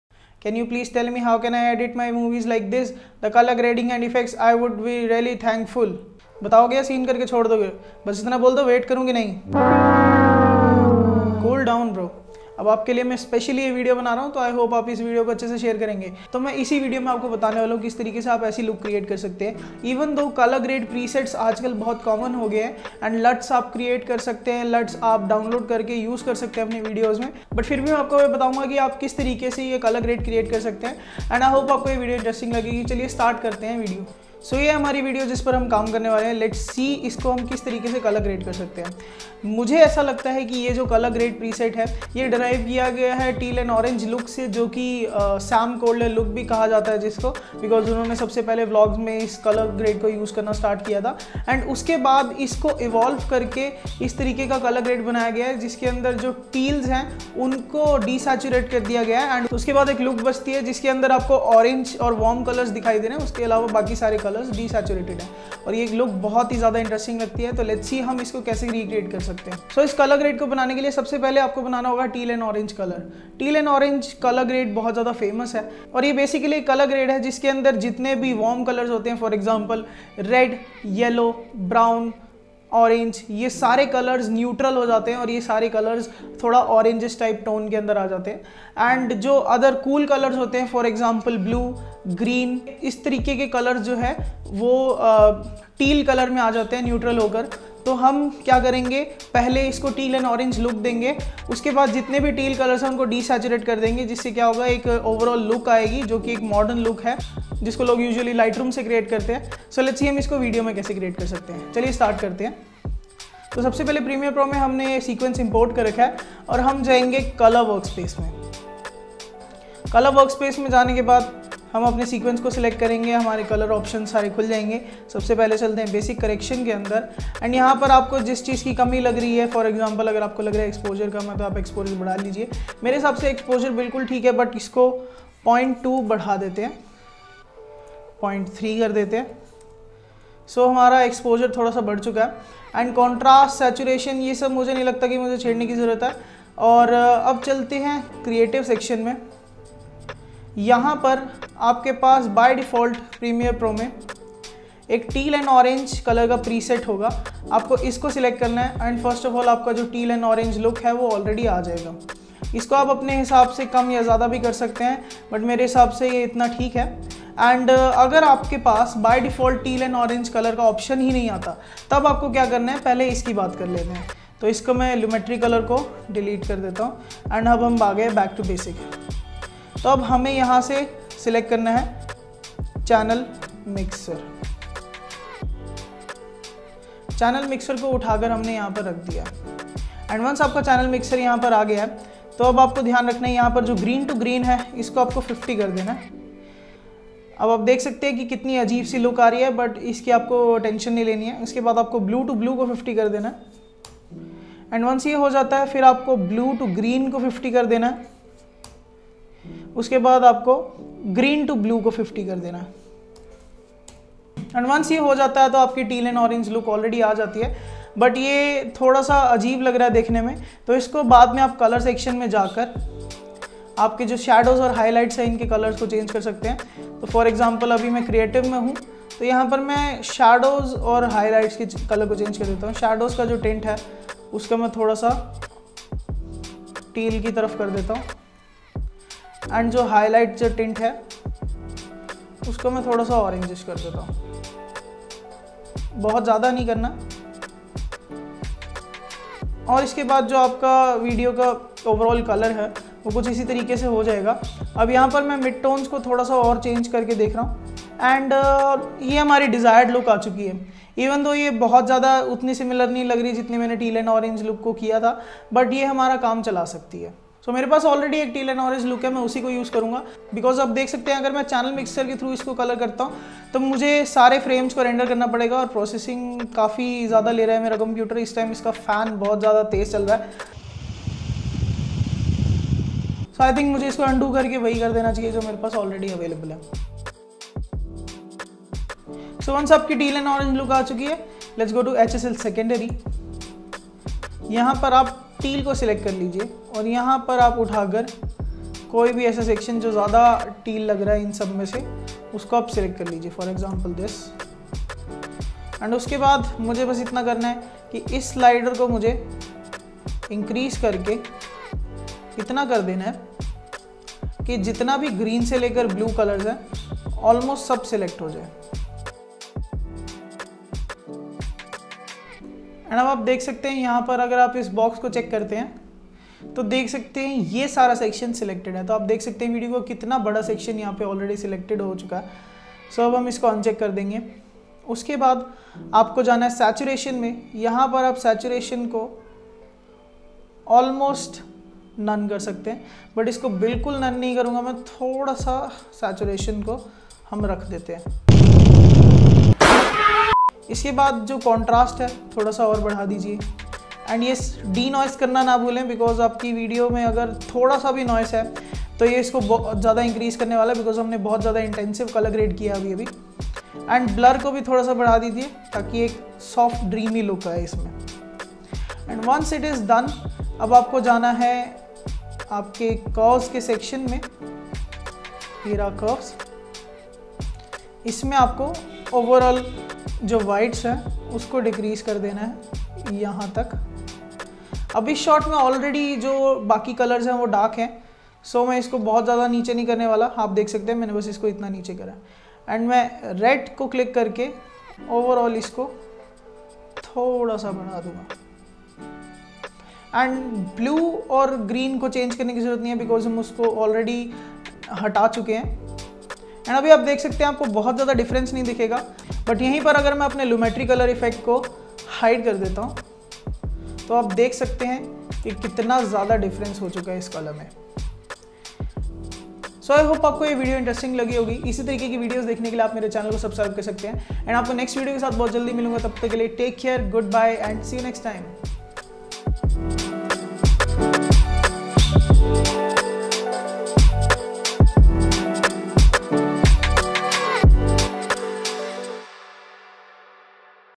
Easily create this amazing Cinematic color grade
This video is a short guide on how to create this mordern cinematic color grade look which was requested by one of my viewers.

Software used for video editing : adobe Premiere Pro
My Personal Equipments :
Phones : SAMSUNG GALAXY M31, VIVO Y83 PRO
Camera : Nikon d3200
DIGITEK Mobile Gimbal
graphics tablet